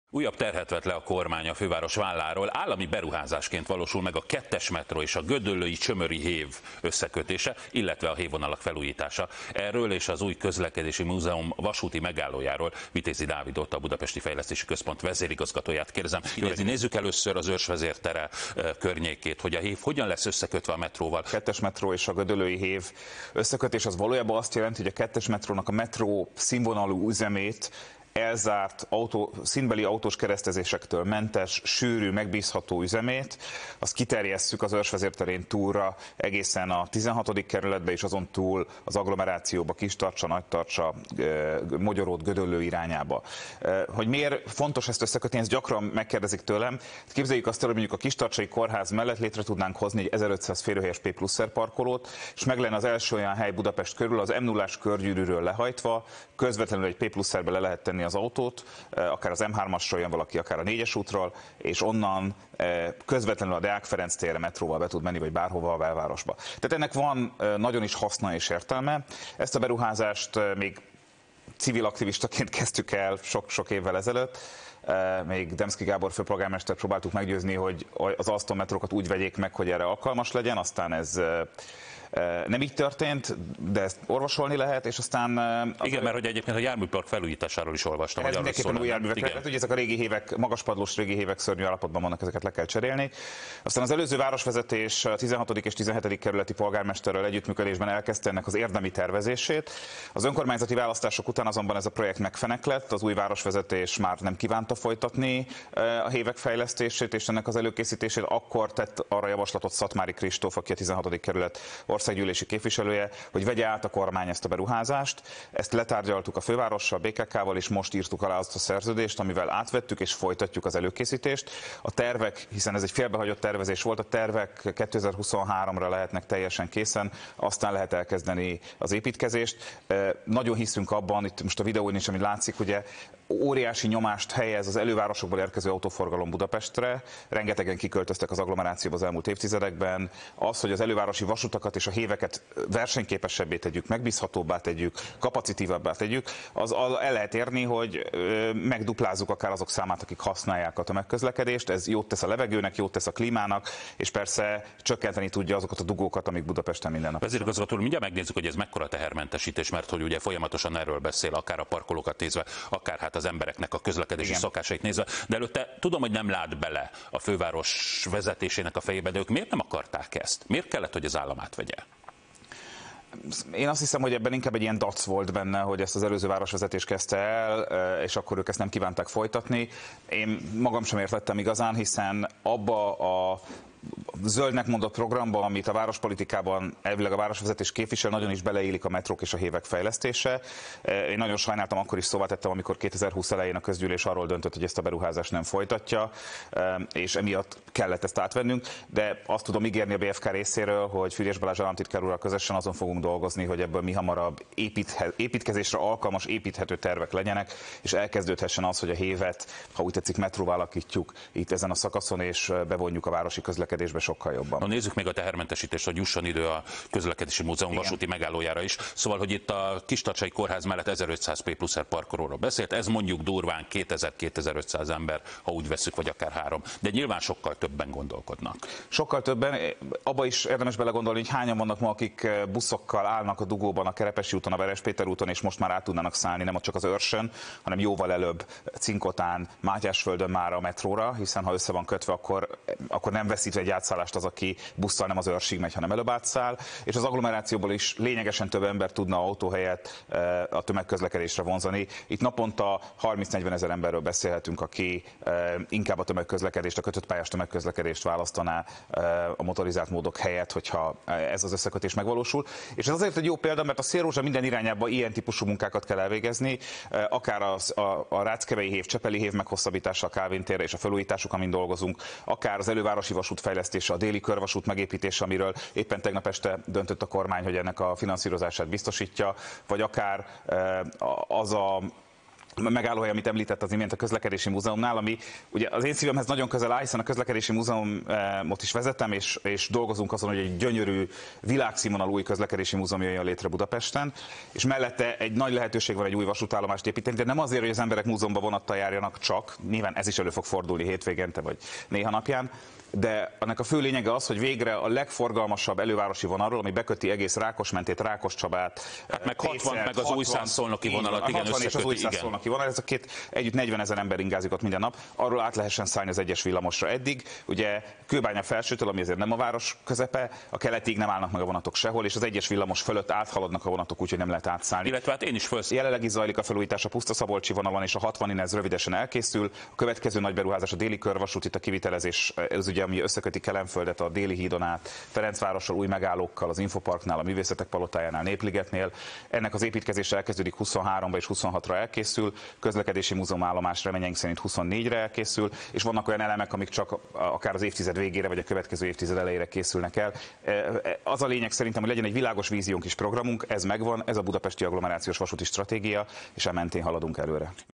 Közös pályán az M2-es metró és a gödöllői hév, új vasúti megálló a Közlekedési Múzeumnál
Az M2-es metró és a gödöllői, valamint a csömöri hévek összekötéséről, a hévvonalak felújításáról, illetve a leendő Közlekedési Múzeumnál épülő új vasúti megállóról beszélgettünk a Mokkában.